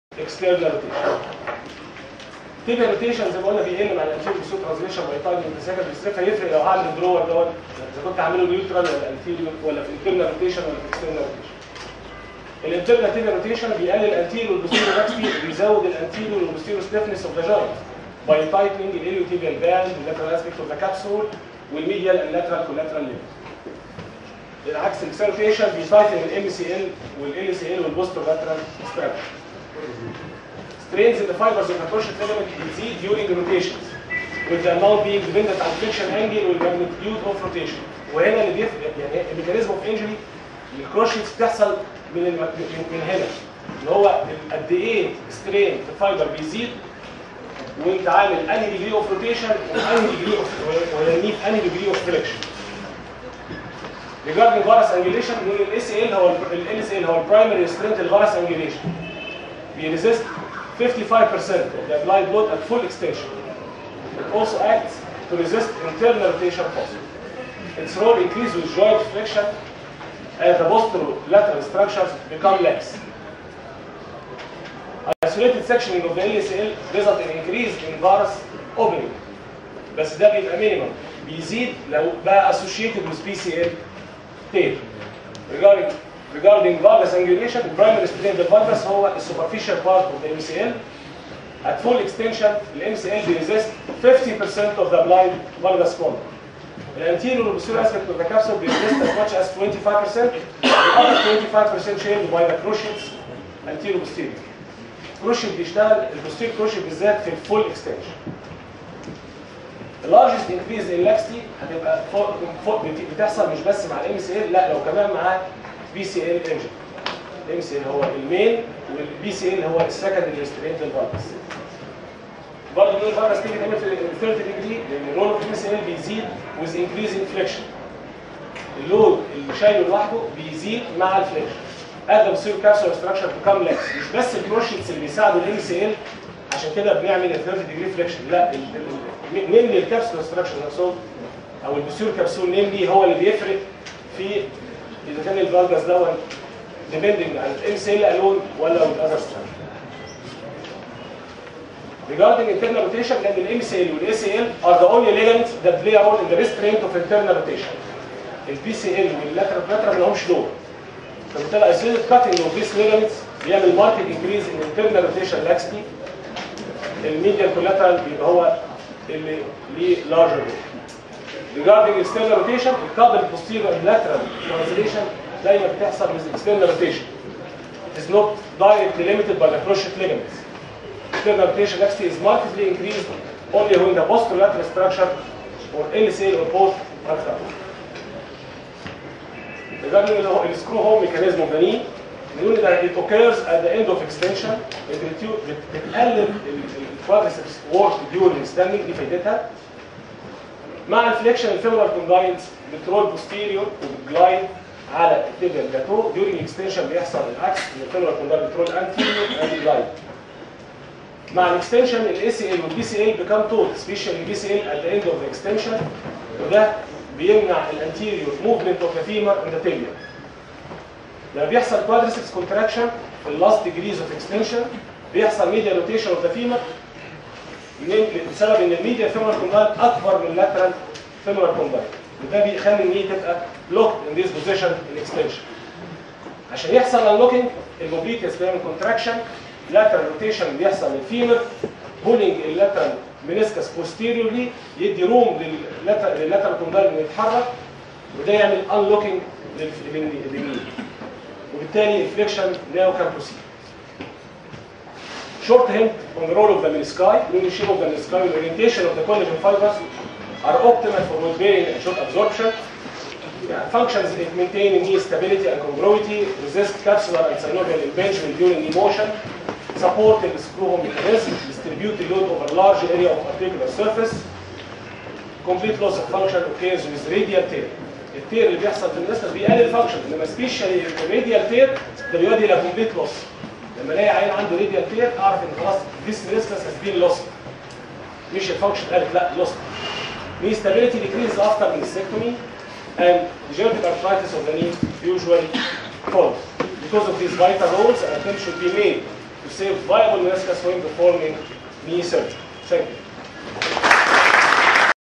Knee joint biomechanics & injury mechanics - D. Ahmed Rizk P2
This is a lecture by D. Ahmed Rizk; Lecturer of orthopaedic surgery - Cairo university (Kasr Al Ainy School of Medicine) which was in "Basic Science in Orthopaedics" held at LRC (Learning Resource Center) in Kasr Al Ainy in February 23, 2012